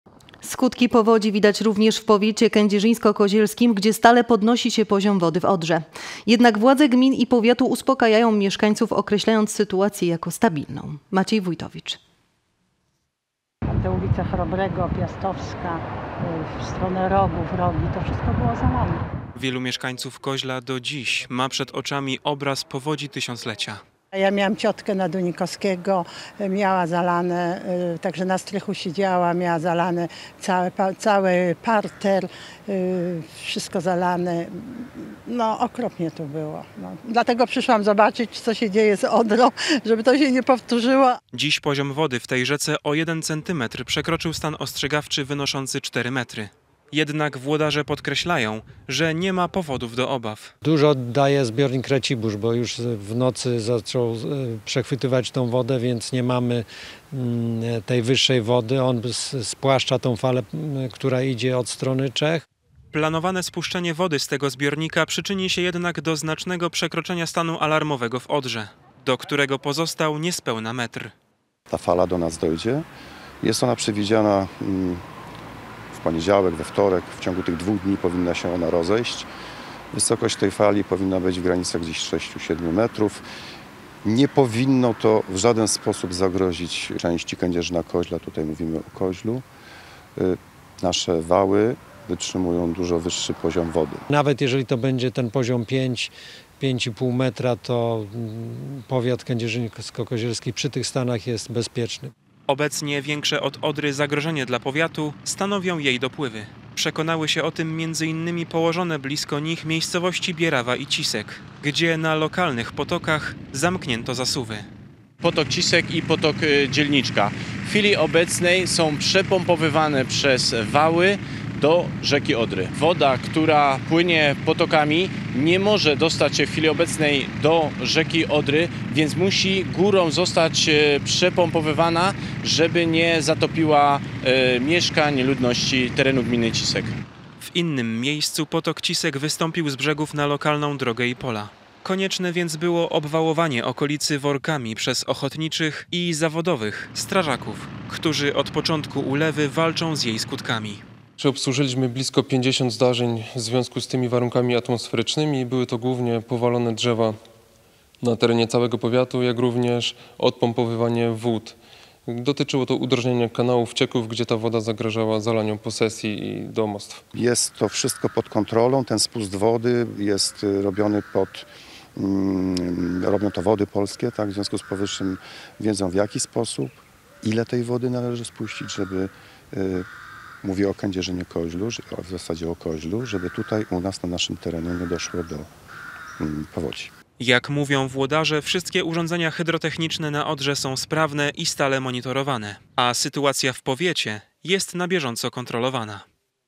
Względny spokój i oczekiwanie na fale. Powiat k-k gotowy na potencjalne skutki ulewy
Skutki powodzi widać również w powiecie kędzierzyńsko- kozielskim, gdzie stale podnosi się poziom wody w Odrze. Jednak władze gmin i powiatu uspokajają mieszkańców określając sytuację jako stabilną.